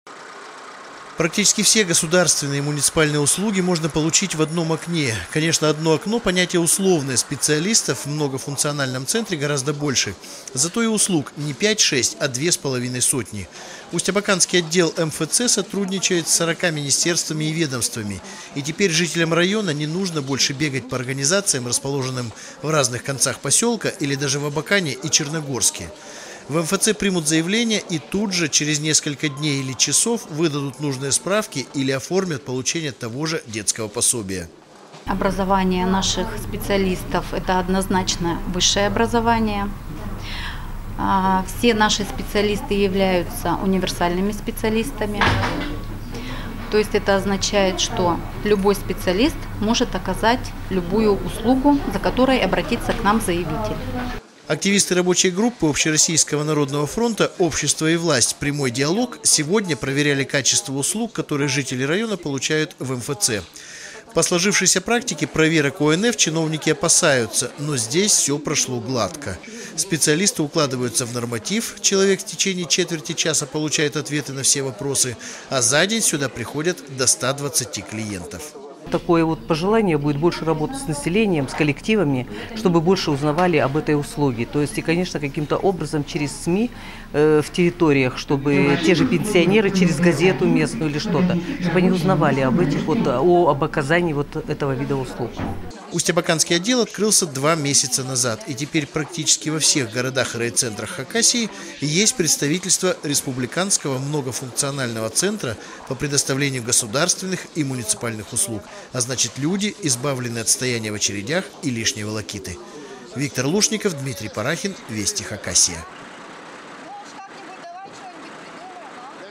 Народный фронт проверил работу многофункционального центра в Усть-Абакане 16.03.2016
По указу Президента , такие   учреждения, работают в каждом крупном населенном пункте. Как обстоят дела в Хакасии.